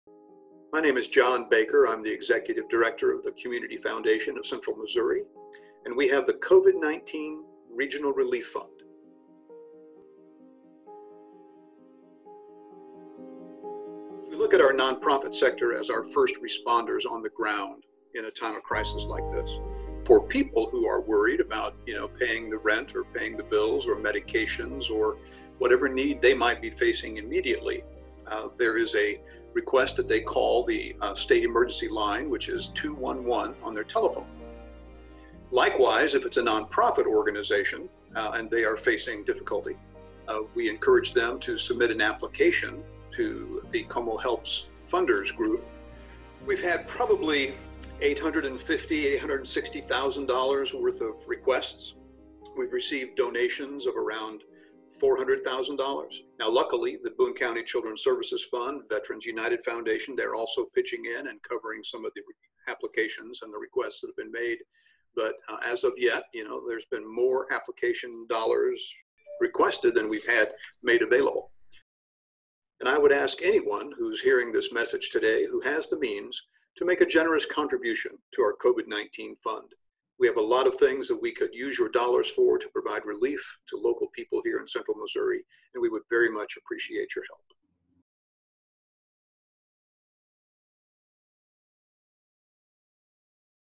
Give Help, Get Help via CoMoHelps.org
CoMoHelps.org launched as a resource hub for giving and seeking aid during the coronavirus pandemic. The project is a joint effort by the City of Columbia, Boone County, the Heart of Missouri United Way, and the Community Foundation of Central Missouri. Along the way additional funders have joined in. Watch this short video on how to request aid or donate to the relief fund.